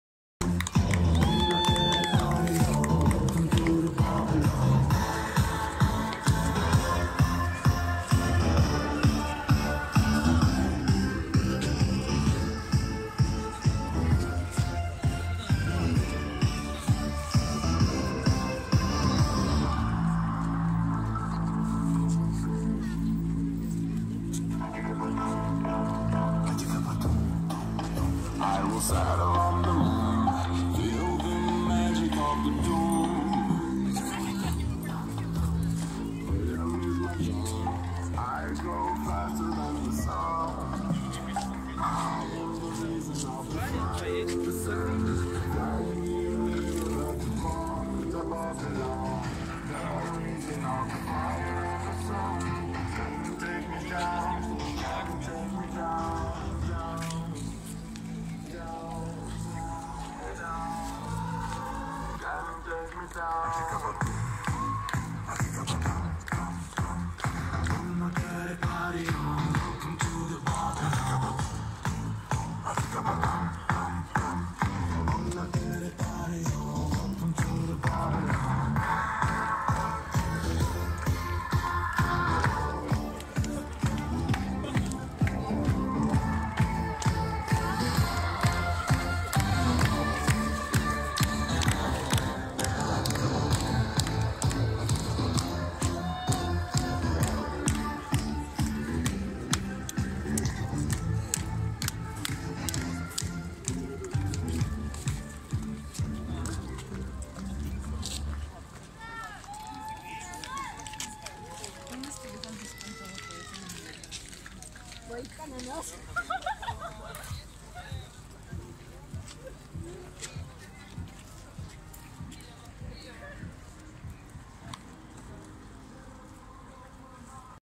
Фаершоу в Петербурге 🔹 уличные артисты на берегу Финского залива 🔹лето 2021🔹
фаершоу спб